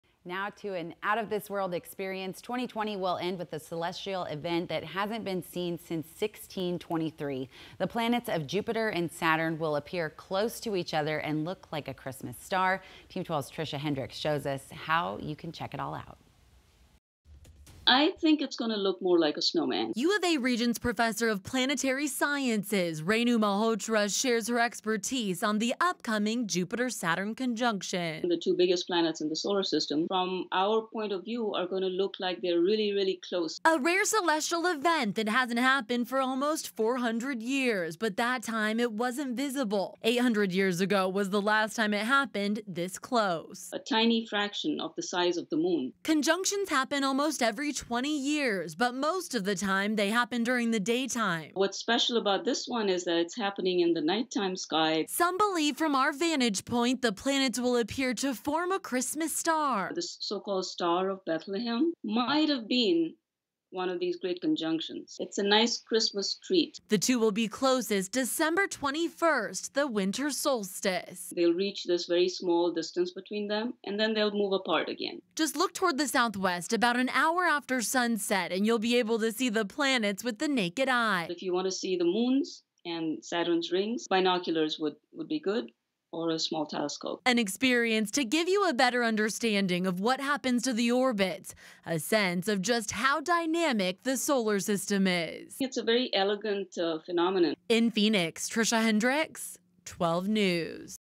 Rare sighting of Jupiter, Saturn alignment
UArizona Regents Professor of Planetary Sciences Renu Malhotra shares her expertise on the upcoming Jupiter Saturn conjunction.
A rare celestial event that hasn't happened for almost 400 years, but at that time it wasn't visible.
800 years ago was the last time it happened this close.